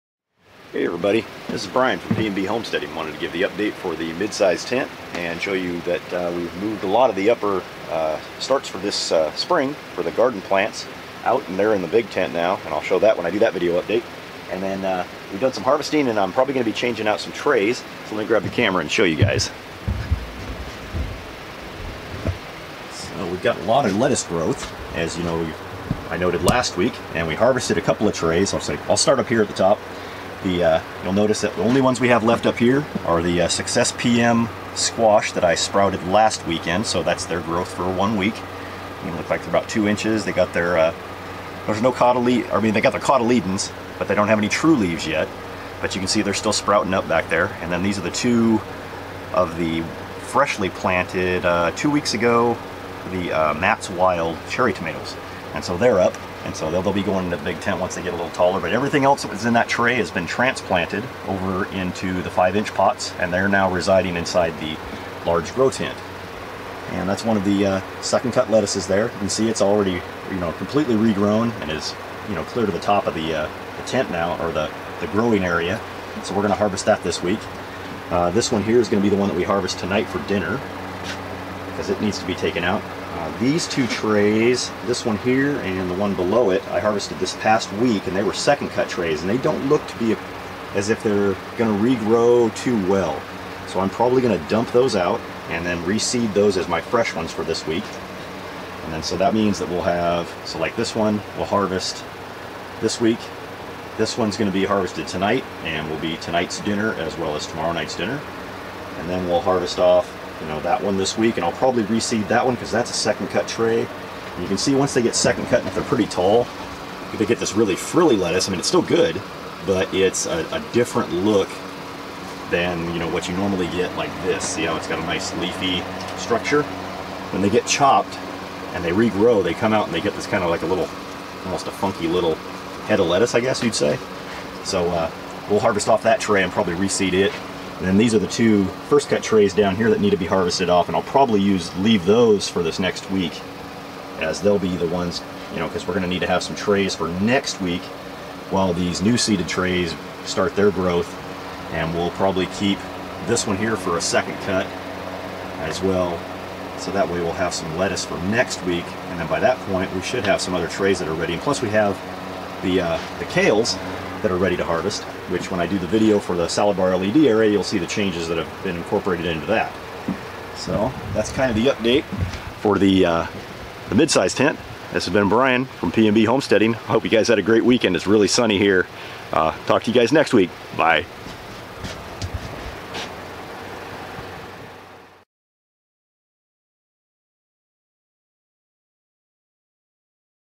Grow tent update for our midsize tent 2018 week 16
Hi Everyone, here's this weeks update for the midsize grow tent. We've moved out most of the spring outdoor garden seedlings to the large grow tent and we're about ready to seed another round of salad green trays. Enjoy the video update! :-)